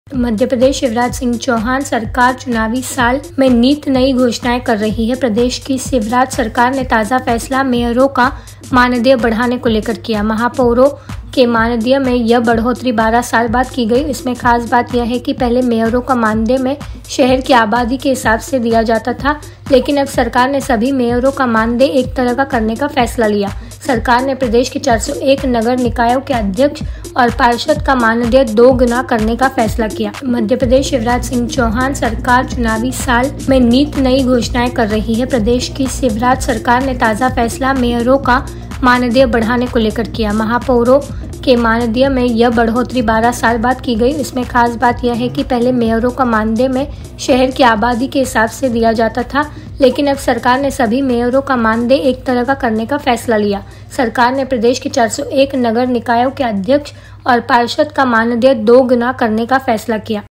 Shivraj Singh Chauhan सरकार मेहरबान हुई, महापौर, अध्यक्ष, उपाध्यक्ष और पार्षदों का मानदेय बढ़ाया
मध्य प्रदेश शिवराज सिंह चौहान (CM Shivraj Singh Chouhan) सरकार चुनावी साल (MP Assembly Election 2023) में नित-नई घोषणाएं कर रही है. प्रदेश की शिवराज सरकार ने ताजा फैसला मेयरों का मानदेय बढ़ाने को लेकर लिया है. महापौरों के मानदेय में यह बढ़ोतरी 12 साल बाद की गई है. इसमें खास बात यह है कि पहले मेयरों का मानदेय में शहर की आबादी के हिसाब से दिया जाता था, लेकिन अब सरकार ने सभी मेयरों का मानदेय एक तरह का करने का फैसला लिया है. सरकार ने प्रदेश के 401 नगर निकायों के अध्यक्ष और पार्षद का मानदेय दोगुना करने का फैसला किया है.

सरकार के इस फैसले से पहले प्रदेश के 16 नगर निगमों के मेयरों का मानदेय अलग-अलग था. इस वजह से भोपाल, इंदौर, जबलपुर और ग्वालियर नगर निगम के मेयर का मानदेय अधिक और 10 लाख से कम आबादी वाले निगमों के मेयर का मानदेय कम था.शिवराज सरकार ने इसे अब एक कर दिया है